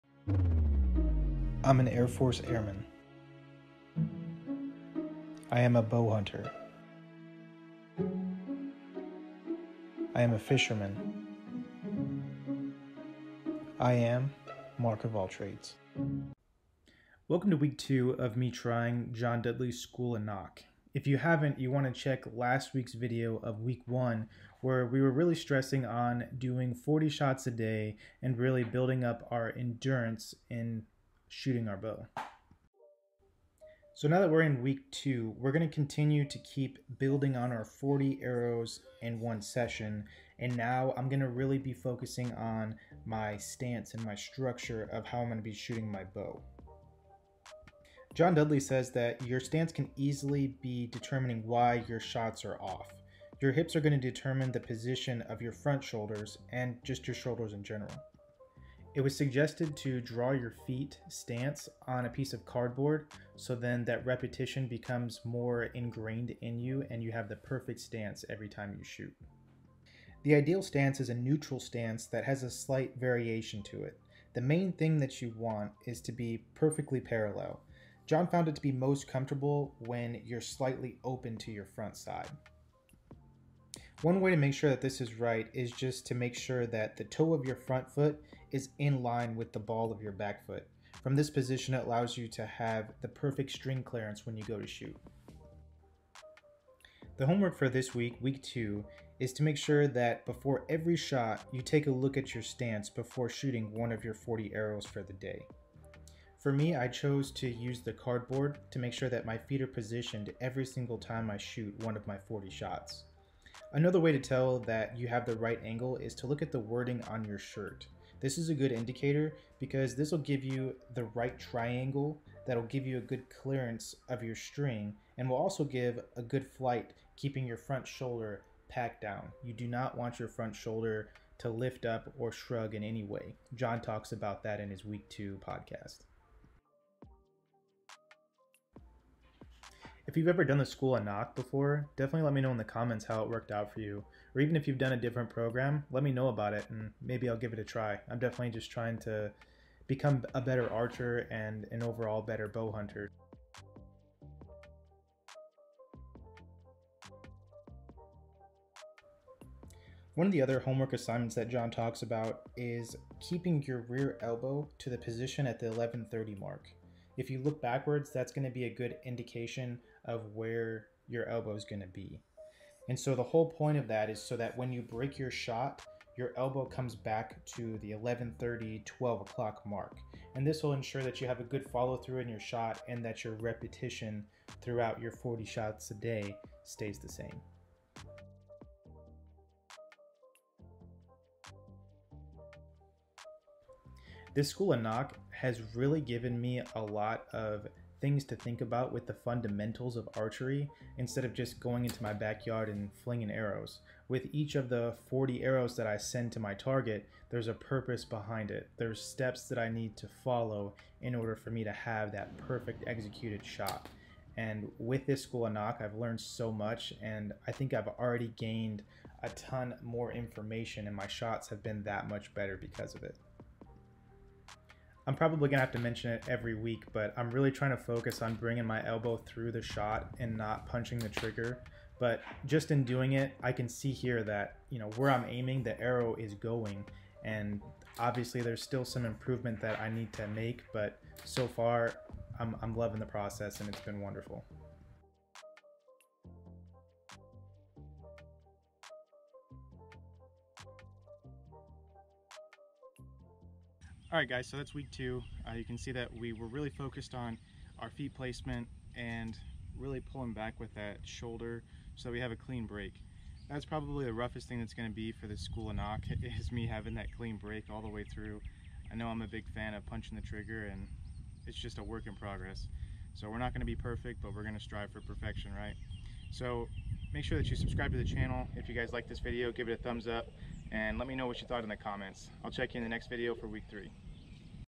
Improve your archery form - Proper Archery Form - School Of Nock Week 2
Today we are going over John Dudley's School of Nock Week 2.  The whole purpose of this week is to build a proper base when shooting archery.  Proper archery form starts with how you position your feet.  I drew my stance on a piece of cardboard so my repetitions would be the same every time I stepped on.  The other thing to note is to always keep your front shoulder down whenever you shoot.  Do not allow the front shoulder to creep up on you.  If that is the case make sure to draw down and start over.  Lastly you want to make sure your thumb release arm is back toward the 11:30 or 12 o'clock position.  This will allow you to have a more repeatable motion whenever you hit the trigger of your thumb release.

This video reflects my own thoughts and does not reflect the opinion of the Air Force or DOD